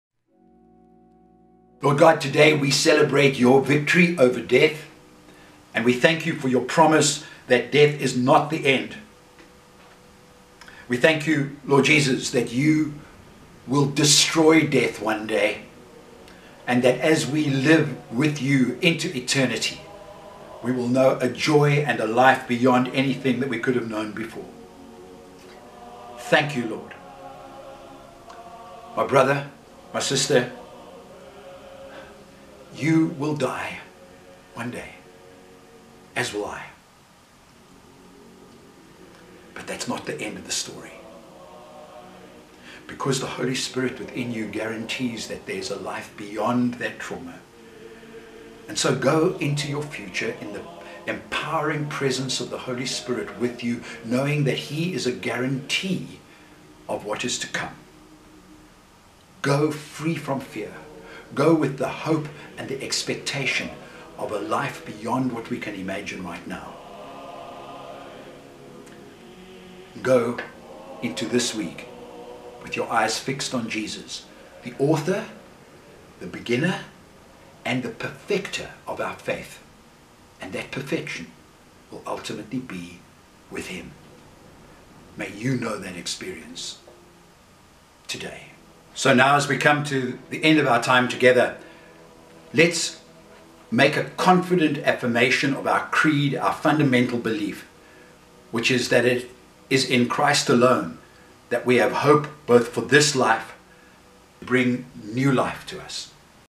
Ken Jackson | 31 May | Closing Prayer
Pastor Ken Jackson, from the United Church Hermanus closes this weeks message in prayer.

ABOUT:

We are an Evangelical, interdenominational Church located in Hermanus that is committed to being a caring and vibrant part of this community. All are welcome.